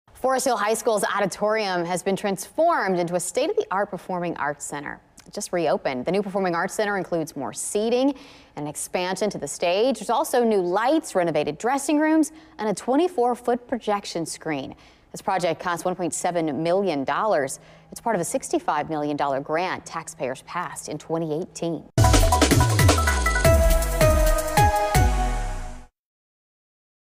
Forest Hill unveils renovations
Forest Hill High School now home to state-of-the-art auditorium